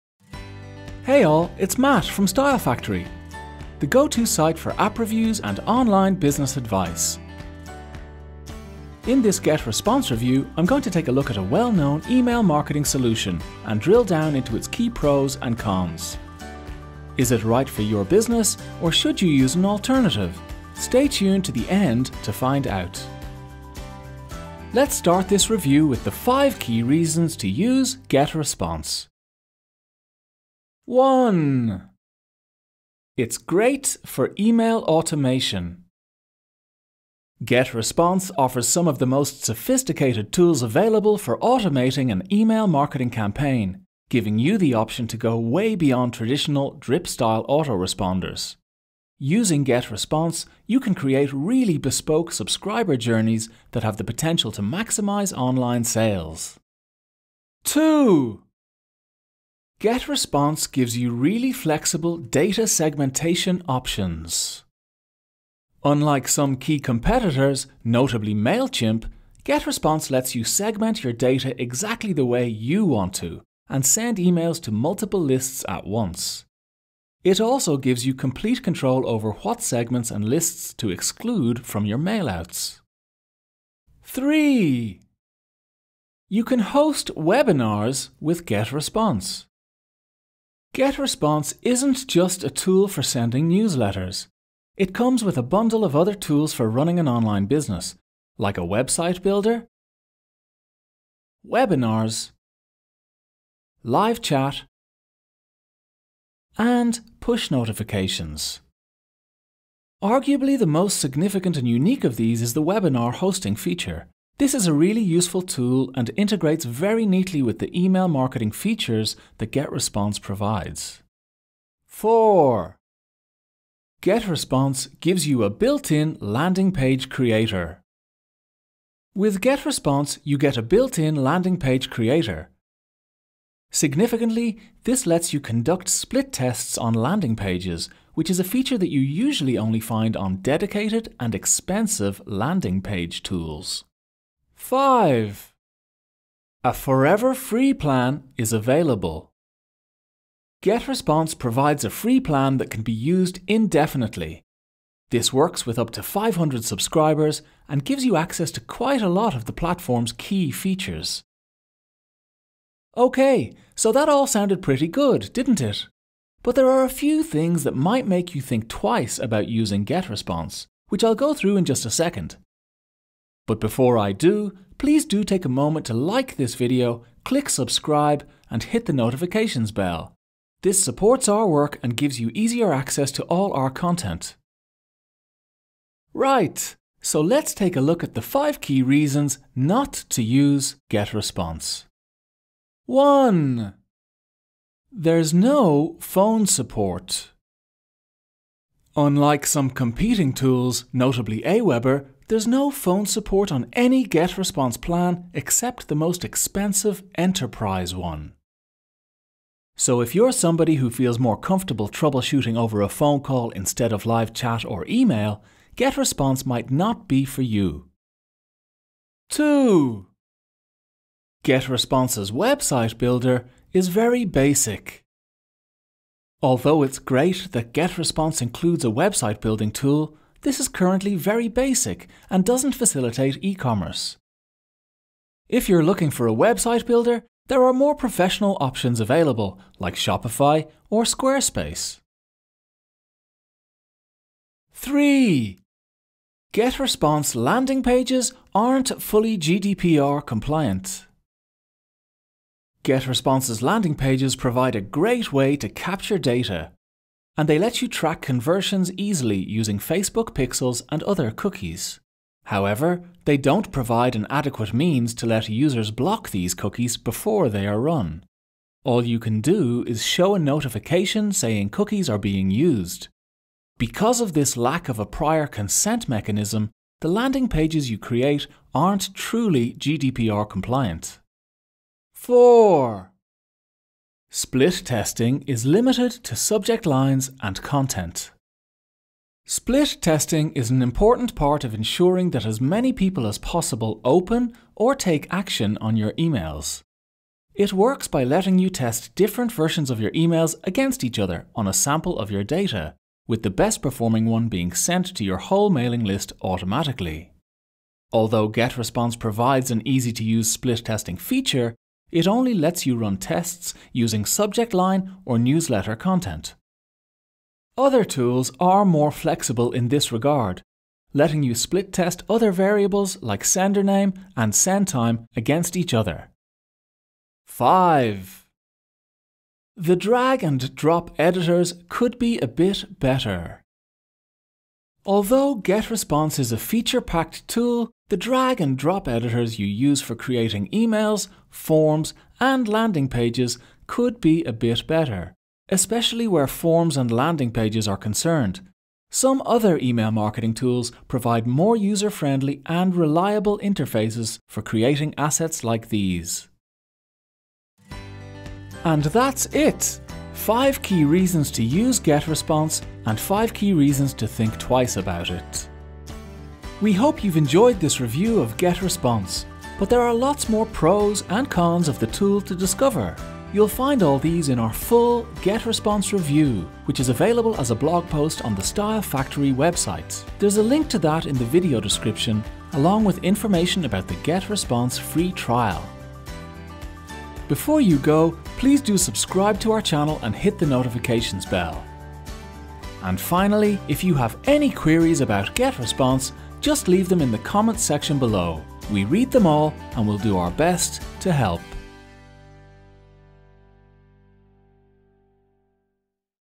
GetResponse Review (2022) — All the Pros and Cons
In this GetResponse review, Style Factory's Matt Walsh takes you through the key advantages and disadvantages of using GetResponse.